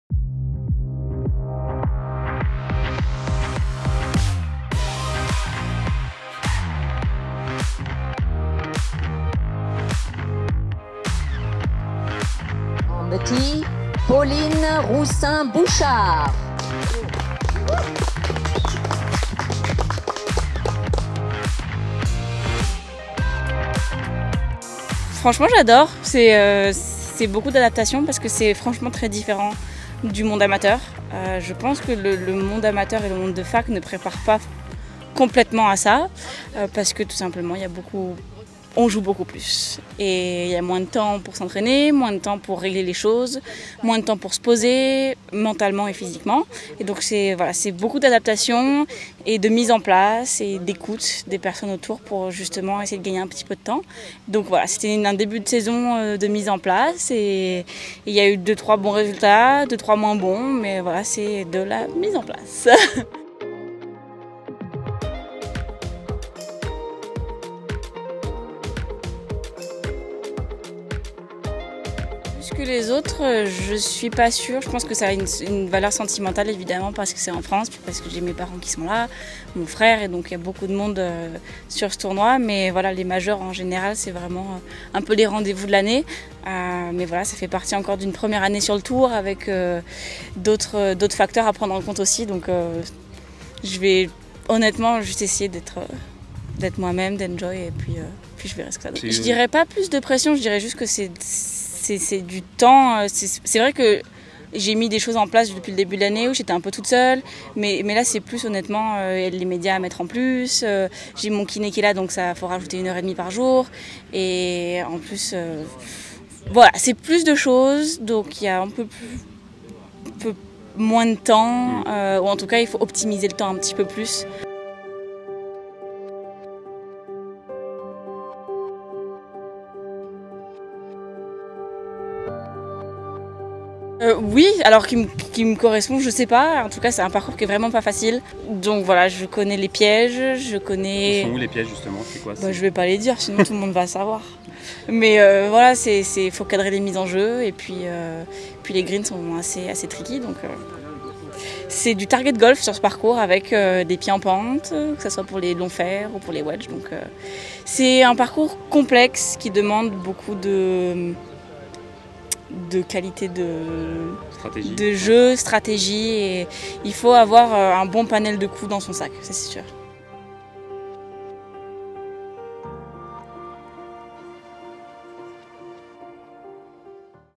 Pauline Roussin-Bouchard heureuse à Evian
Pauline Roussin-Bouchard dispute cette année son 3e Evian Championship, le premier en tant que membre du LPGA. Une première saison où la Française s'est offert pour le moment 2 top 10 et pointe au 147e rang mondial. Une année sous le signe de l'adaptation.